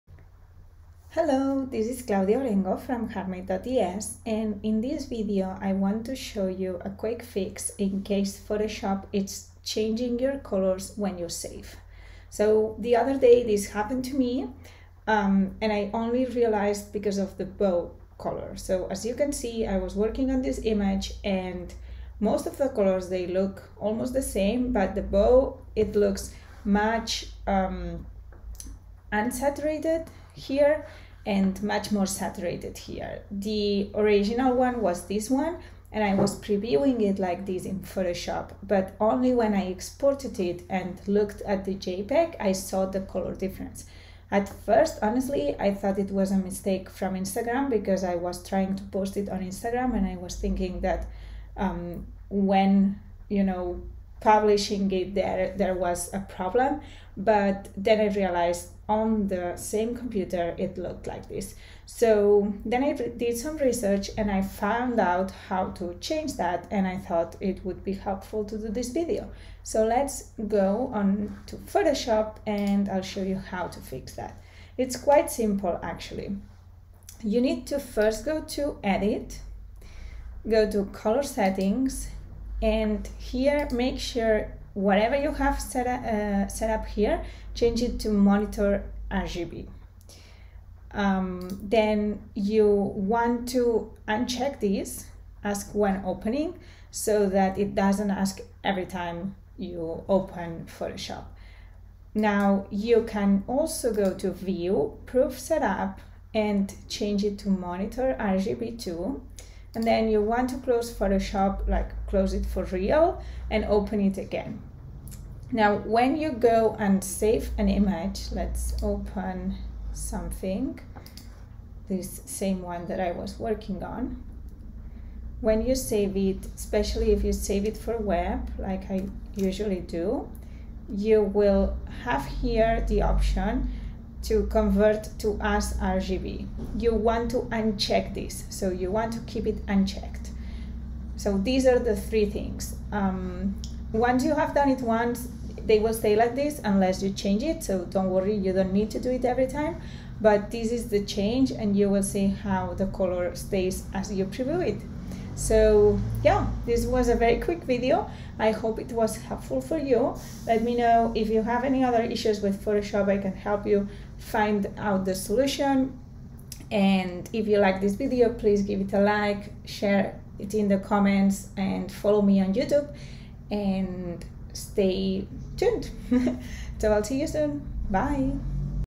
Photoshop "save for web" changing color. How to solve the error easily.
Hiii!
The other day I noticed that when I was saving images from Photoshop some colors changed from what I previewed to what was exported, specially when using the "save for web" function. I did some research and discovered that this error was happening because of the Photoshop color settings.

In this video I'm showing you how to solve the Photoshop color changing problem with a very quick and easy solution.